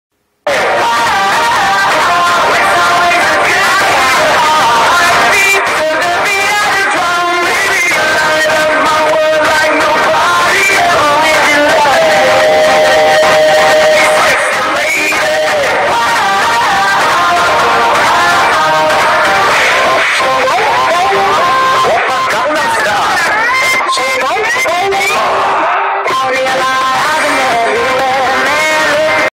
Best Soccer/Football Kids Skills & Shot by Kamron *HD* (2/09/2013)
Best Soccer Kids Skills & Shot.This is Kamron Habibullah.only 9 years old! Playing U 12